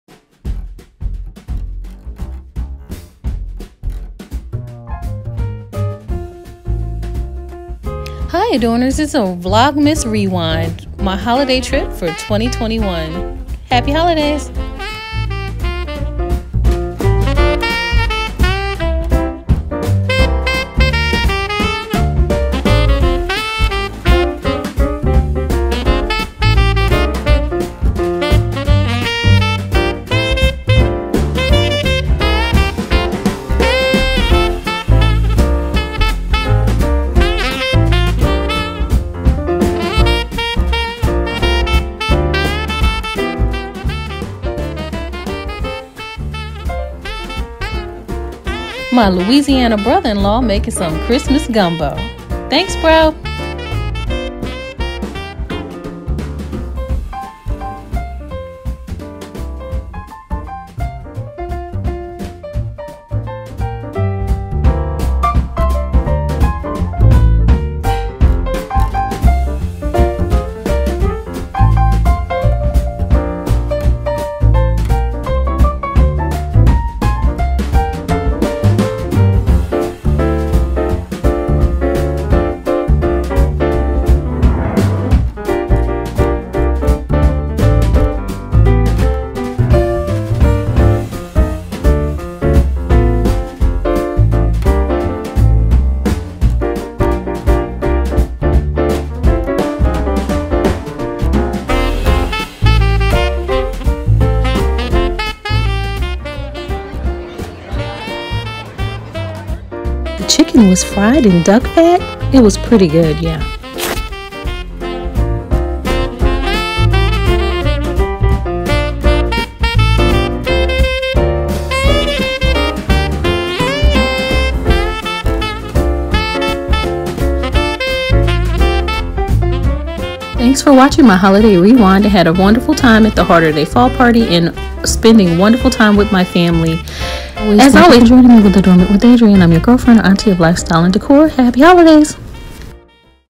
Holiday Rewind! Vlogmas 2021 Holiday Trip and Celebrations!
Hi Adorners,
Just a short vlogmas video sharing my 2021 holiday season! While I didn’t include photos of my family members you can be sure that they made my holiday amazing! Christmas lights, ice tubing , gumbo and more! How do you spend your holiday?
Happy holidays,
Adrienne 🤶🏾🤶🏼🎄🛷❤️🔆✨